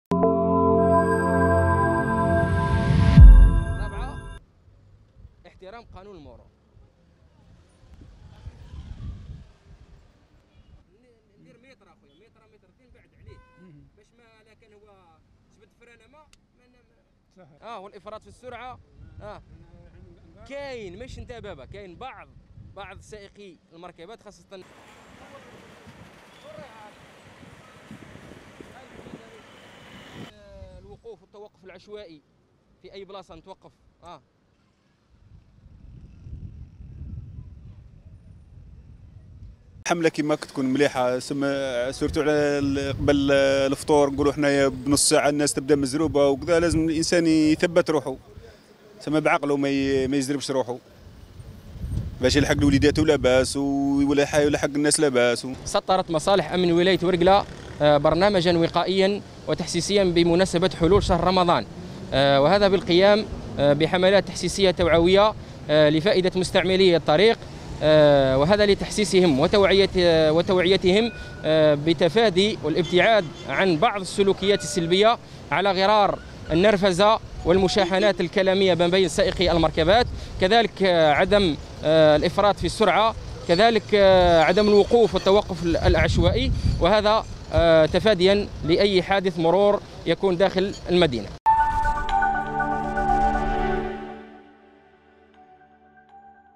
ورقلة : اطلاق حملة تحسيسية للوقاية من حوادث المرور خلال شهر رمضان
ورقلة (وأج)- سطرت مصالح امن ولاية ورقلة برنامجا وقائيا وتحسيسيا بمناسبة حلول شهر رمضان، تضمن القيام بحملات تحسيسية لفائدة مستعملي الطريق، بهدف توعيتهم باهمية تفادي بعض السلوكيات السلبية على غرار النرفزة و المشاحنات الكلامية بين السائقين، اضافة الى عدم الافراط في السرعة، سعيا الى تقليص عدد الحوادث المرورية خلال الشهر الفضيل.